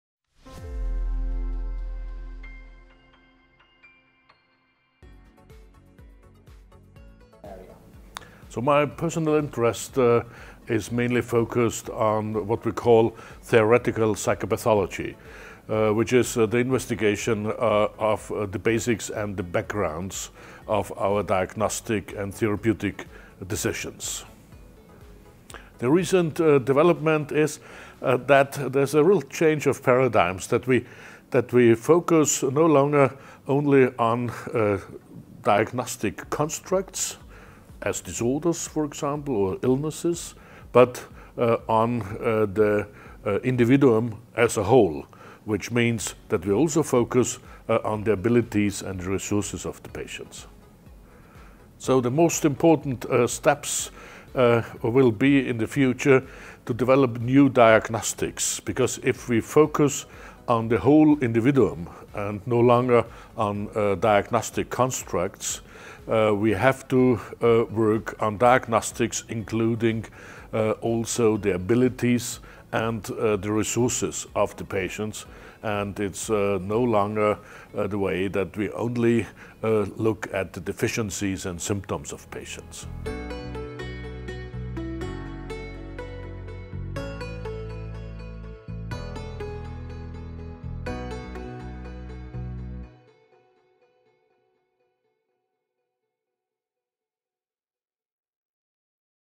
What has been a recent development in theoretical psychopathology?
Professor Michael Musalek discusses recent developments in theoretical psychopathology.